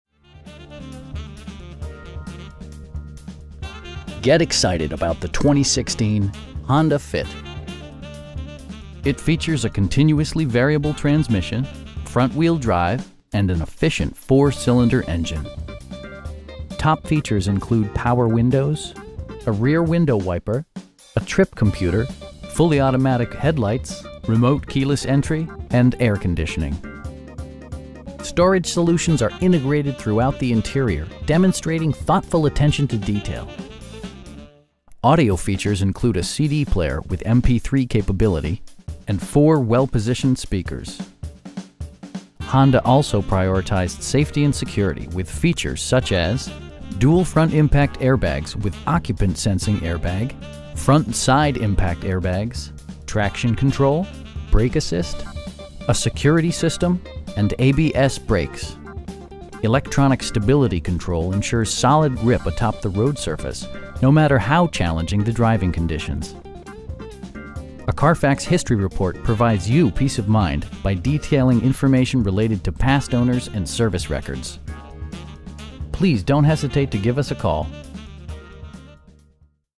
2016 Honda Fit LX in Soquel, CA 95073
Ocean Honda

Get excited about the 2016 Honda Fit. It features a continuously variable transmission, front-wheel drive, and an efficient 4 cylinder engine. Top features include power windows, a rear window wiper, a trip computer, front bucket seats, fully automatic headlights, telescoping steering wheel, remote keyless entry, and air conditioning. Storage solutions are integrated throughout the interior, demonstrating thoughtful attention to detail. Audio features include a CD player with MP3 capability, and 4 speakers, providing excellent sound throughout the cabin. Honda also prioritized safety and security with features such as: dual front impact airbags with occupant sensing airbag, front side impact airbags, traction control, brake assist, a security system, and ABS brakes. Electronic stability control ensures solid grip atop the road surface, no matter how challenging the driving conditions. A Carfax history report provides you peace of mind by detailing information related to past owners and service records. Our aim is to provide our customers with the best prices and service at all times. Please don't hesitate to give us a call.